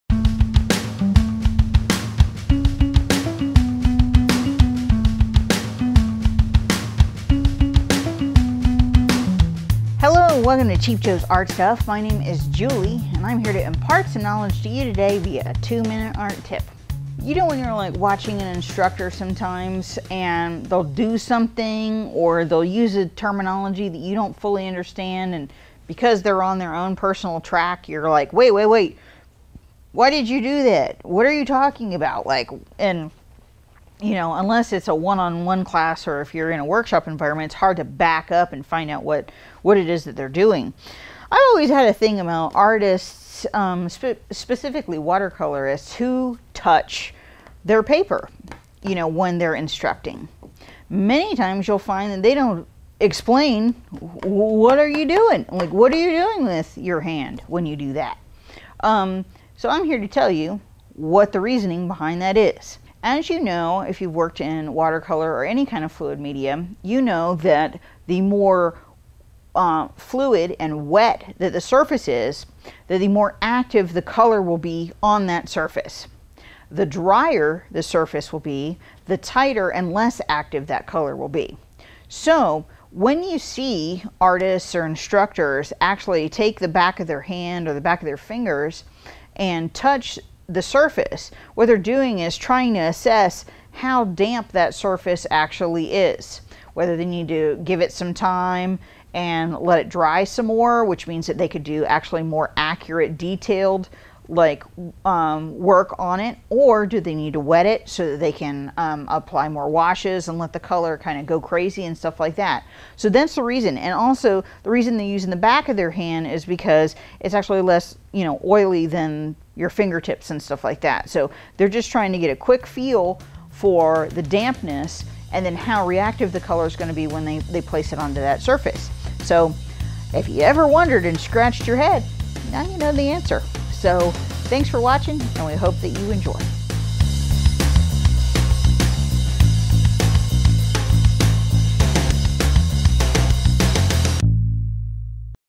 Cheap Joe's 2 Minute Art Tips - Why Do Watercolor Artists Touch Their Paper
In this absolutely free 2 Minute Art Tip, Julie tells you why watercolor artists often touch their paper.